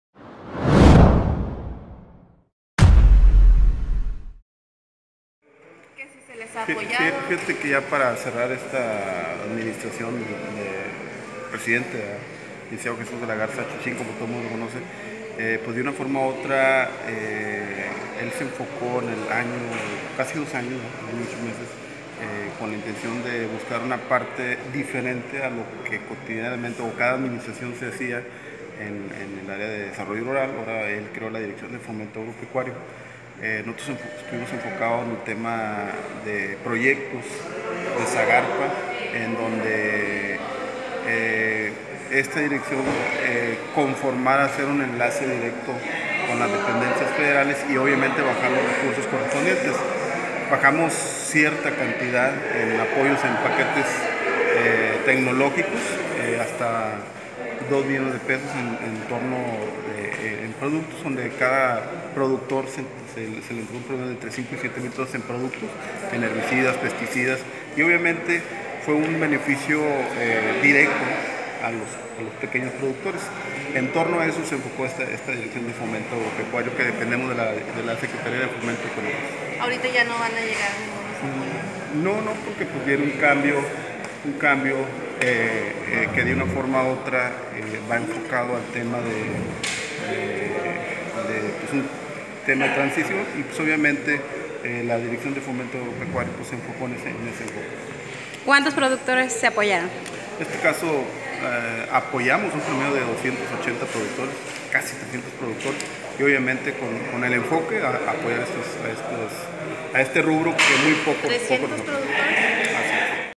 Llegan apoyos para productores
Julio Villarreal Zamorano titular de la Secretaría Fomento Agropecuario señaló que aproximadamente 280 productores fueron  favorecidos con los apoyos que gestionaron ante  la Sagarpa